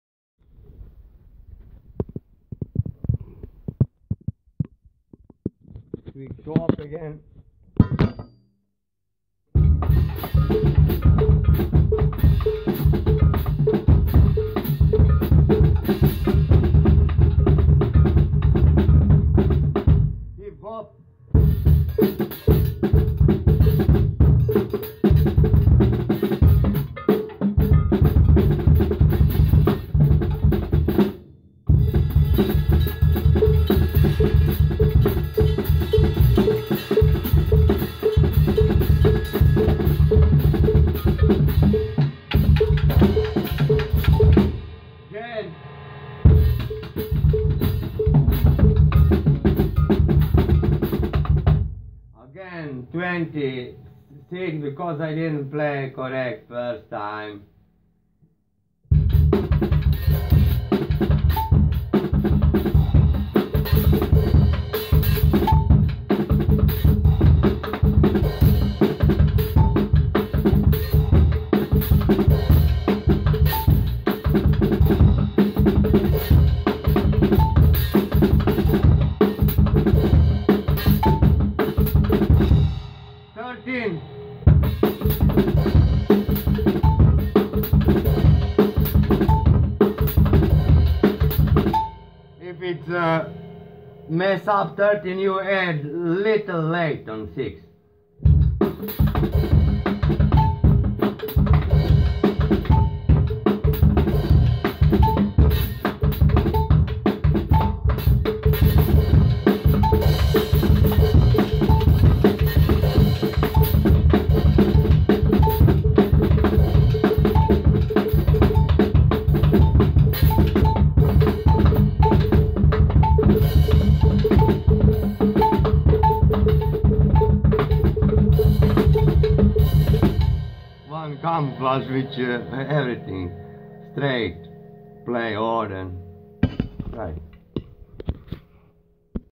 SHORTLY I DISCOVER NEW POLY CAMOUFLGE GREAT ISN IT I SHITTING THAT I KNOW FROM BEFORE:)
WOW EB-METHOD PHILOSPHY UNBEATABLE DEVELOPING TALENT TEHNIQUE IS WORK TALENT IS WISDOM TO QUCKLY DEVELOP TALENT VERY SIMPLE 3 DEGREE IS WHEN FOUR LIMBS PLAYING DIFFERENTLY AND 3 AND MORE PEDAL CONSTANTLY MOVING 4 DEGREE AJAJ 2 BARS EVERY LIMB AND 4 AND MORE PEDALS CONSTANTLY MOVE IT SOUND EASY BUT NEVER ANYBODI BEEN ON 4 DEGREE I SHOWING RESULTS I DONT PRACTICE SO MUCH!5 DAYS MAYBE 3 DEGREE PROPERLY AND ALREADY JUMP HIGH HA?I BE PUTTING LANGIANA THAT IS MOST IMPORTANT TO ME THAT I BECOMING BEST NO FIRST IAM INTERESTED IN SOUND MANIPULATIONS TODAY I INVENT FROM SIMPLE CROSSOVER TECHNOLOGY ALREADY ON FACE LATER TILL TOMMOROW HERE A?